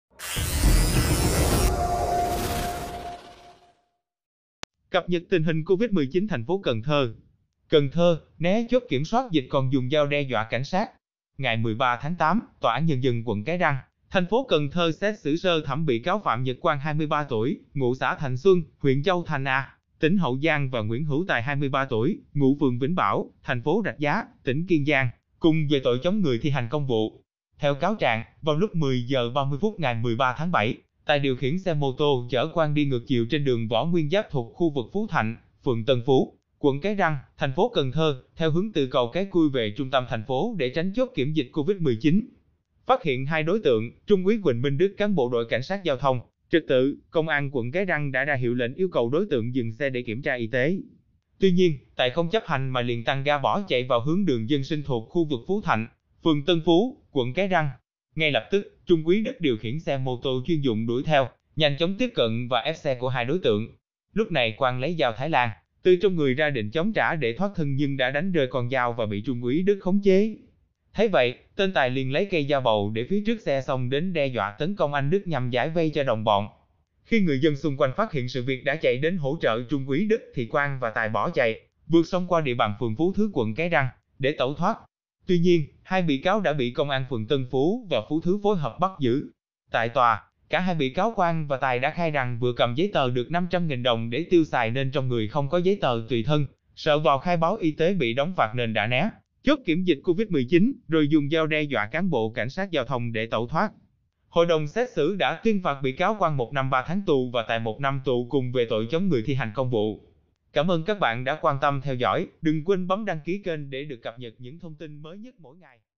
Cần Thơ:🔥 Né Chốt Kiểm Soát Dịch, 2 Thanh Niên Dùng Dao Tấn Công Cảnh Sát. Lãnh Án Tù
Cần Thơ Quận Bình Thủy Phong Tỏa, thông tin chi tiết ở video nha mọi người